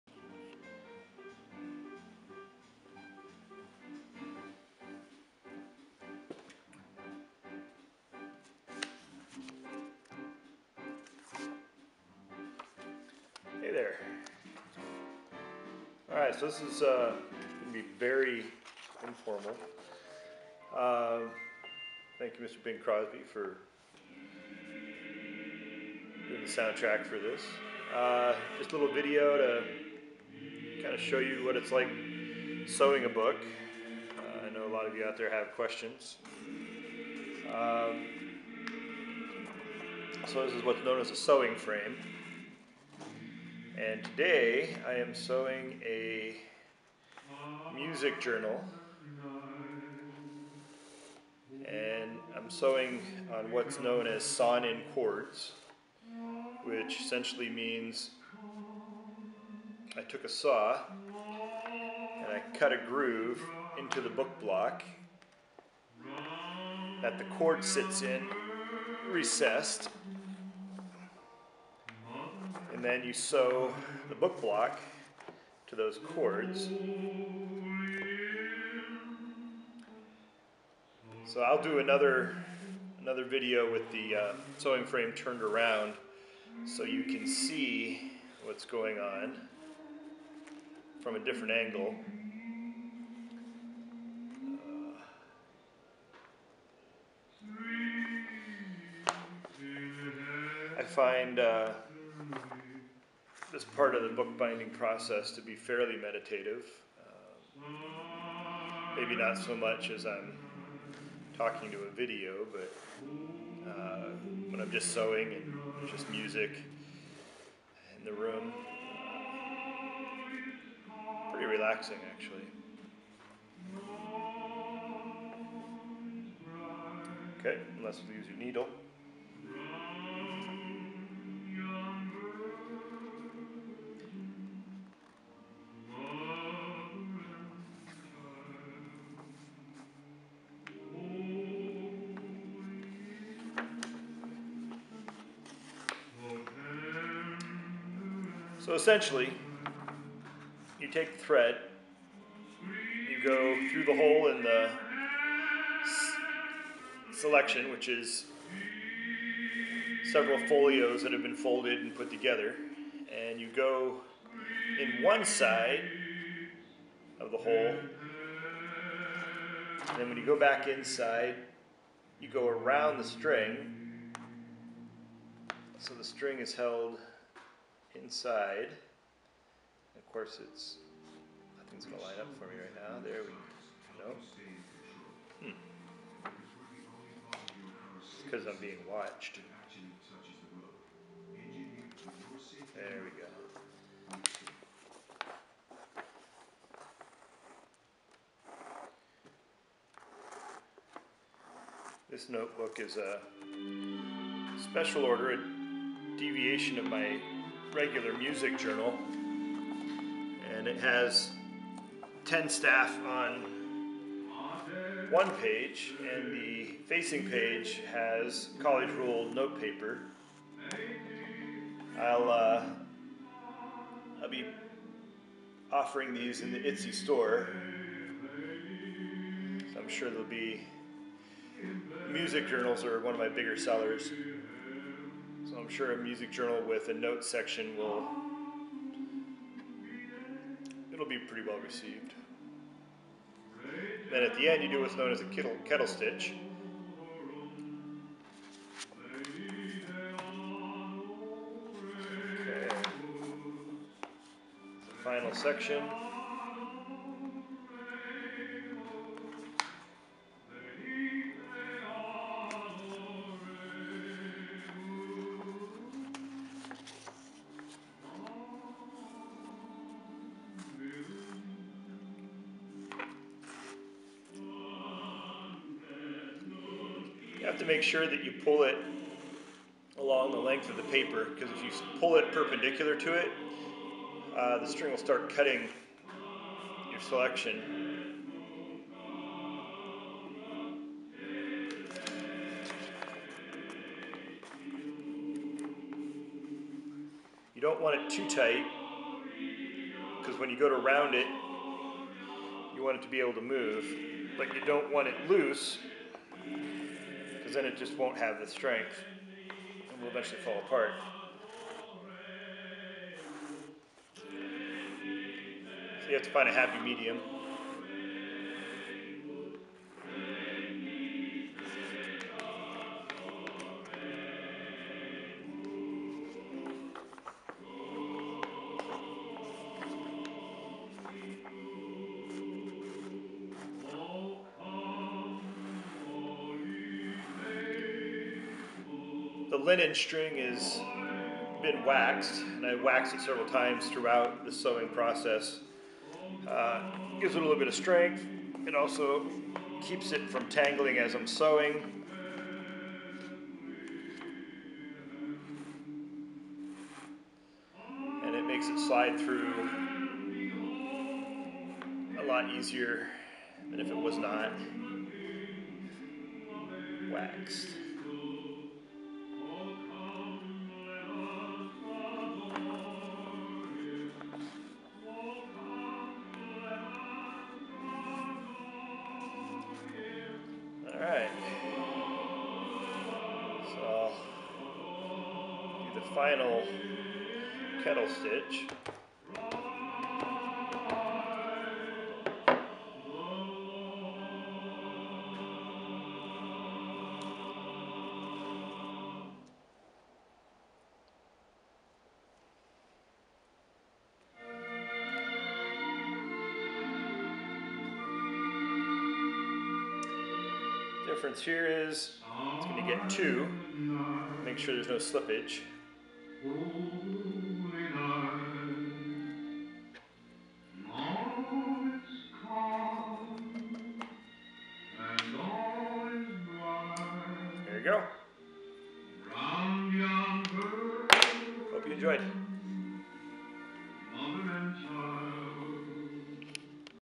Informal Sewing of Sawn-In Cords Bookbinding
Just an informal "bookbinding tutorial" of sewing sawn-in cords. Really it's just me sewing a book and talking a bit to you. I'll post another video from the other direction so you can see the spin of the book as I sew. At some point I would like to do a quality bookbinding tutorial series but that is a solid year or so off.